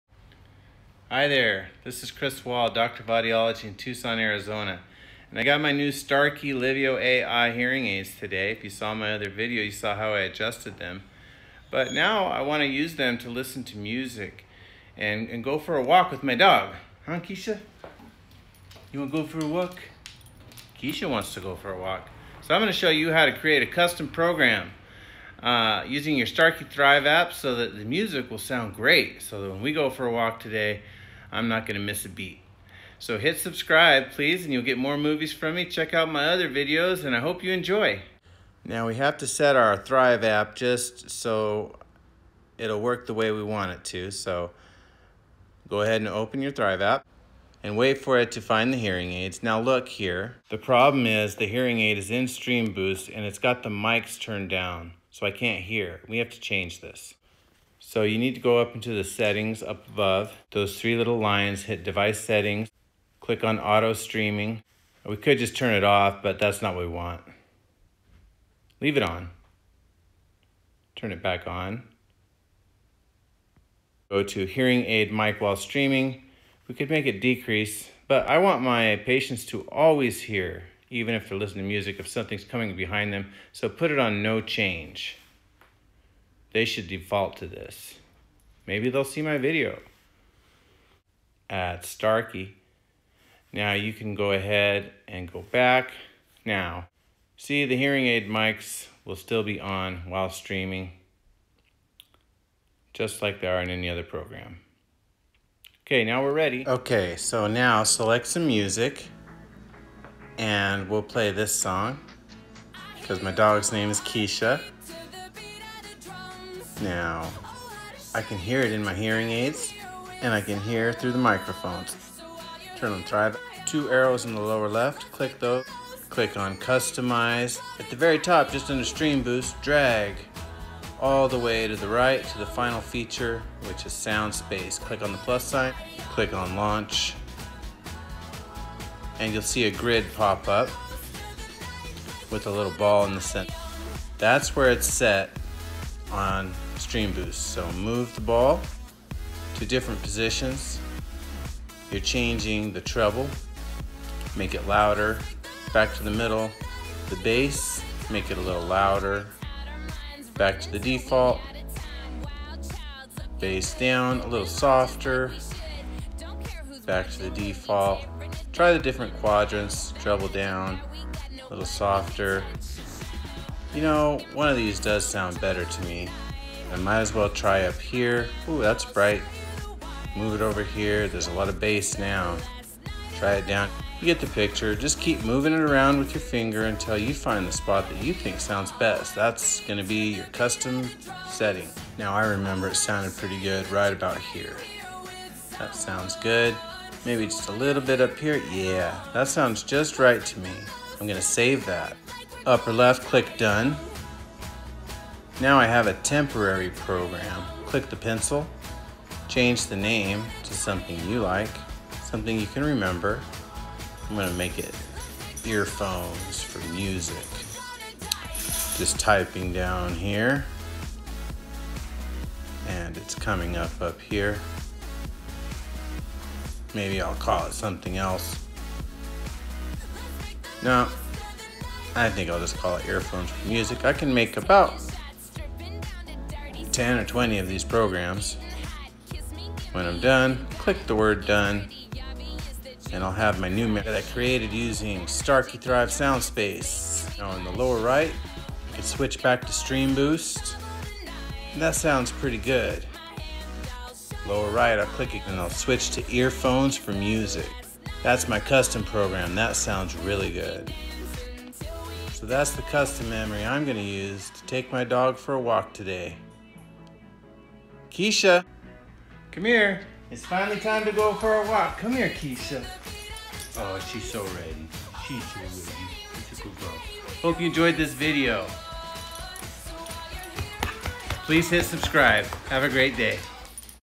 Create Custom Music Programs for Starkey Livio AI Bluetooth Hearing Aids
This video shows you how to create a custom music program in the Starkey Thrive Smartphone App to enhance the sound quality of the music you stream from your smartphone to your Bluetooth hearing aids.